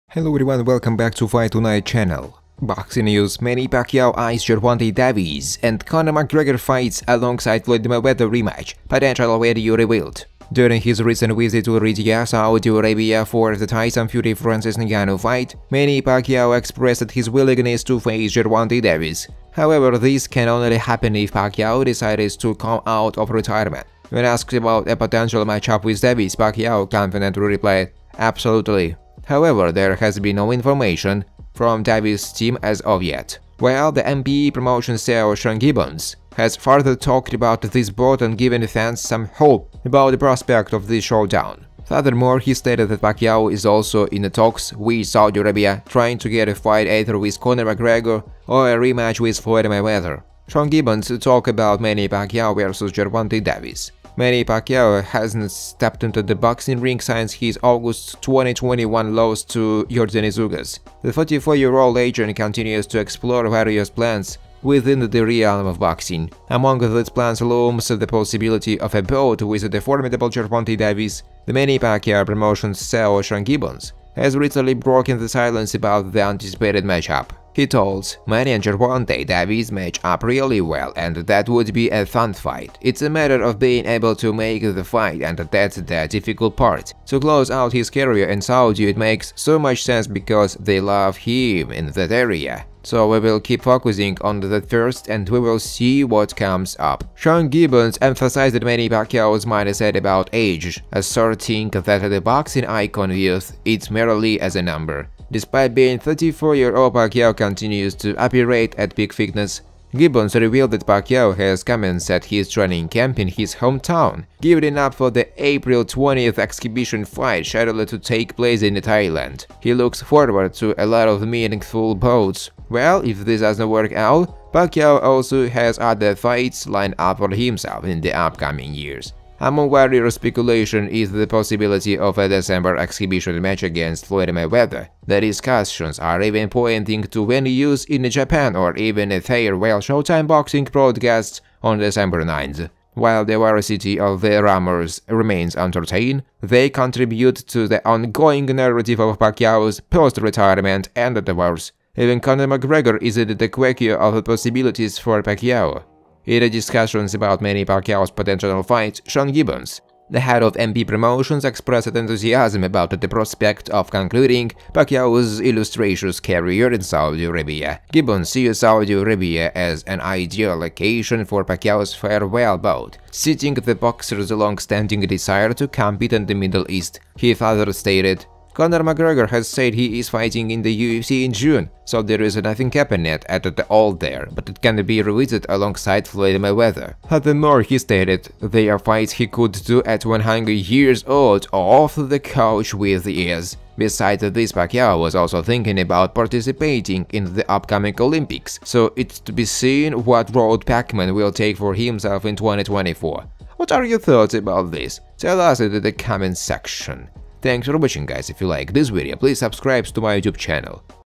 DANGEROUS! Manny Pacquiao Finally ACCEPTS Gervonta Davis FIGHT CHALLENGE
Sean Gibbons emphasized Manny Pacquiao’s mindset about age, asserting that the boxing icon views it merely as a number. Despite being 44 years old, Pacquiao continues to operate at peak fitness. Gibbons revealed that Pacquiao has commenced his training camp in his hometown, gearing up for the April 20th exhibition fight scheduled to take place in Thailand. He looks forward to a lot of meaningful bouts. Well, if this doesn’t work out, Pacquiao also has other fights lined up for himself in the upcoming year.

Among various speculations is the possibility of a December exhibition match against Floyd Mayweather. The discussions are even pointing to venues in Japan or even a farewell Showtime boxing broadcast on December 9. While the veracity of these rumors remains uncertain, they contribute to the ongoing narrative of Pacquiao’s post-retirement endeavors. Even Conor McGregor is in the queue of possibilities for Pacquiao.

In discussions about Manny Pacquiao’s potential fights, Sean Gibbons, President of MP Promotions, expressed enthusiasm about the prospect of concluding Pacquiao’s illustrious career in Saudi Arabia. Gibbons sees Saudi Arabia as an ideal location for Pacquiao’s farewell bout, citing the boxer’s long-standing desire to compete in the Middle East. He further stated, “Conor McGregor has said he is fighting in the UFC in June so there’s nothing happening at all there, but it can be revisited alongside Floyd Mayweather.”

in a surprising turn of events, boxing legend Manny Pacquiao may be considering a return to the ring. The Filipino maestro, who hung up his gloves in 2021 after a defeat against Yordenis Ugas, could potentially face the undefeated Gervonta ‘Tank’ Davis for a dramatic finale in Saudi Arabia, a nation where Pacquiao has a sizeable fan base. Sean Gibbons, the manager of Pacquiao’s promotional company, is steering these discussions with enthusiasm, viewing Davis as the perfect adversary for Pacquiao’s potential swan song.

A Potential Showdown on the Horizon
While Pacquiao’s current focus is on exhibition matches, notably an upcoming bout in Thailand scheduled for April 20, he has shown openness towards a sanctioned fight if the opportunity is compelling. Gibbons maintains that despite Pacquiao’s age, the boxing great is still in top form. He is set to commence training in his hometown, with the April exhibition match serving as a stepping stone for a significant event in the summer.

Discussions Underway with Saudi Arabian Sporting Authorities
Gibbons is in ongoing discussions with Saudi Arabian sporting authority figures about the potential fight, including his Excellency Turki Alalshikh. The plan is to make the Pacquiao-Davis fight a grand spectacle, capitalizing on Pacquiao’s popularity in the region. Gibbons did not entirely rule out potential bouts against Conor McGregor and Floyd Mayweather, although these matchups are viewed as less likely. McGregor’s UFC commitments pose a challenge, and the ease with which Pacquiao could face Mayweather even at an advanced age makes the Davis fight a more tantalizing prospect.

Davis is represented by Premier Boxing Champions who are known for keeping the vast majority of their promotions in-house. This habit was snapped with Tank’s most recent fight, which was a seventh-round knockout of the Golden Boy-promoted Ryan Garcia last April. As exciting as this was, it was viewed as something of an aberration rather than a watershed moment.

Also, PBC recently struck a deal with Amazon Prime following Showtime’s departure from pro boxing. Would they be willing to stake one of their biggest names against a promotional rival this early in a new business relationship?

Haney is not under contract with any promoter, but his most recent outing – a dominant 12-round unanimous decision win over Regis Prograis – was presented by Matchroom Boxing.

The 25-year-old Haney is now the reigning WBC super lightweight champion and the former undisputed lightweight king. You’d think potential big-name opponents would be kicking his door down, right? Wrong!

Sean Gibbons, who runs Manny Pacquiao's promotional company, believes the Filipino superstar would be very interested in a showdown with unbeaten puncher Gervonta 'Tank' Davis.

Pacquiao, 45-years-old, retired from the sport in 2021 after suffering a twelve round decision loss at the hands of then welterweight champion Yordenis Ugas.

Gibbons explains that the sporting authority in Saudi Arabia is very interested in having Pacquiao fight in the country - with the contest being the final fight of his career.

The industry veteran views 'Tank' as a great opponent for his boxer.